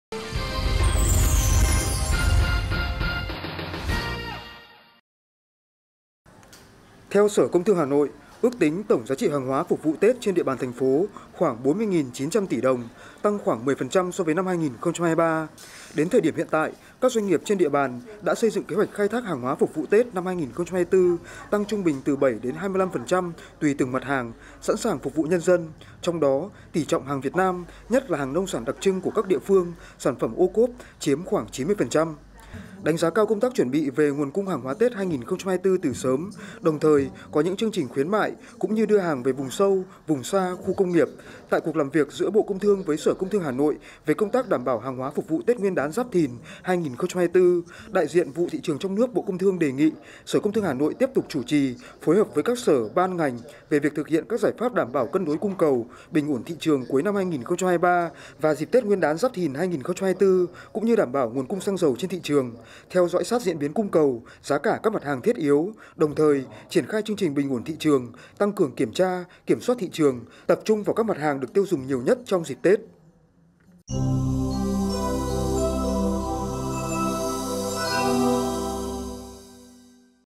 Hà Nội chủ động đảm bảo nhu cầu tiêu dùng dịp Tết năm 2024
Theo Sở Công Thương Hà Nội, ước tính tổng giá trị hàng hóa phục vụ Tết trên địa bàn thành phố khoảng 40.900 tỷ đồng, tăng khoảng 10% so với năm 2023. Đến thời điểm hiện tại, các doanh nghiệp trên địa bàn đã xây dựng kế hoạch khai thác hàng hóa phục vụ Tết năm 2024 tăng trung bình từ 7 – 25% tùy từng mặt hàng, sẵn sàng phục vụ nhân dân, trong đó, tỷ trọng hàng Việt Nam, nhất là hàng nông sản đặc trưng của các địa phương, sản phẩm OCOP chiếm khoảng 90%...